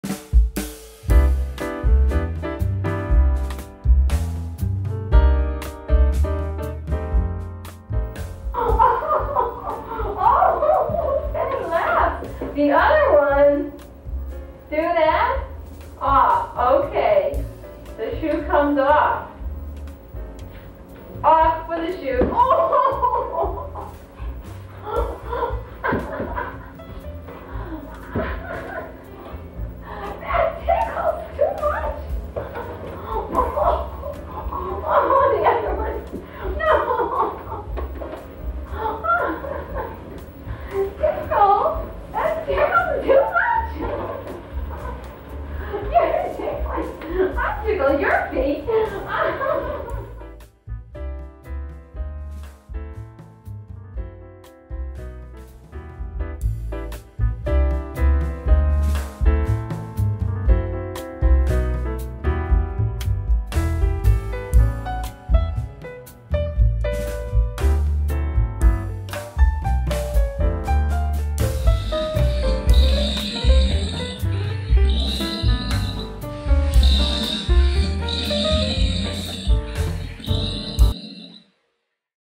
Koko Tickles Penny's Foot
In this video, Koko tickles Penny’s left foot, then signs to Penny to remove her other shoe so Koko can tickle Penny’s right foot. Gorillas, like many other primates, engage in tickling as a form of play and social bonding. Tickling can be a way for gorillas to communicate and establish social relationships with others, including other gorillas as well as humans. Koko had a very close relationship with Penny and would often engage in playful behaviors with her. Koko tickled Penny's foot as a playful gesture; she laughs and smiles afterward. Gorillas are known to have a sense of humor and enjoy making others laugh, and tickling can be a way for them to do so.